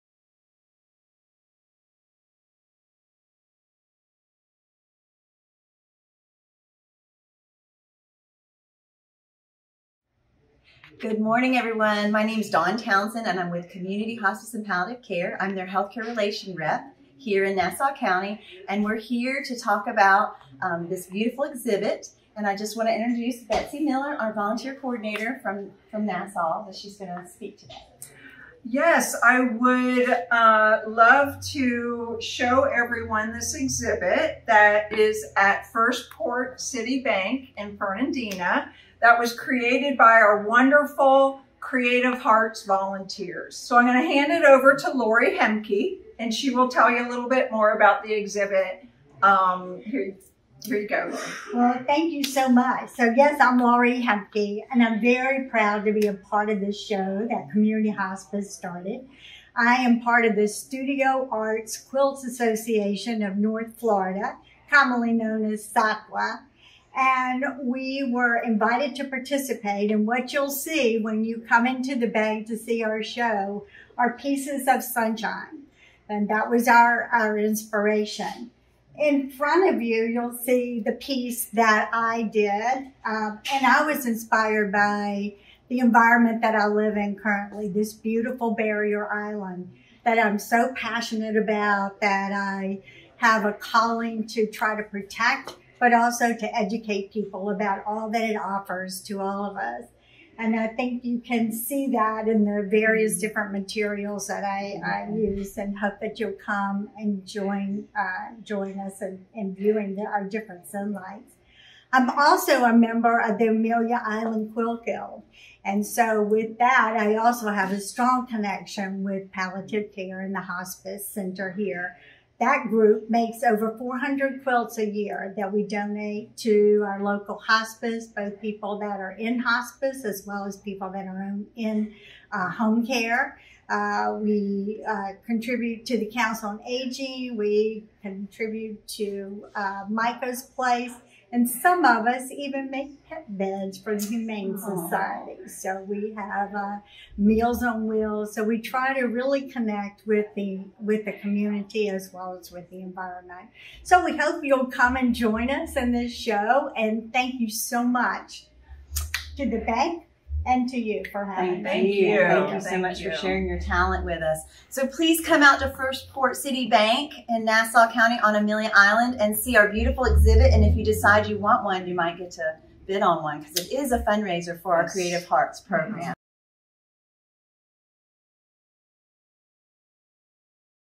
Community Hospice & Palliative Care - Creative HeARTS program: Quilt Exhibit
The Cover Me In Sunshine art quilt exhibit was created for Community Hospice & Palliative Care by the Studio Art Quilt Associates (SAQA) North Florida Group. The sun theme was selected because of the universal symbol of light and love. The exhibit will be displayed at locations throughout our 16-county service area after which these beautiful quilts will be available for purchase in January. Proceeds benefit the Creative HeARTS program Memory Pillow Project. Memory Pillows are special keepsakes created by Community Hospice volunteers for families of patients in our care. Made from an article of clothing owned by the patient, this complimentary pillow is a one-of-a-kind treasure and loving memento.

“Our hope is that people can take a few moments to view and enjoy the exhibit. We are delighted that these art quilts will help raise funds to support Creative HeARTS program.”

-Lisa Hamilton, SAQA North Florida Group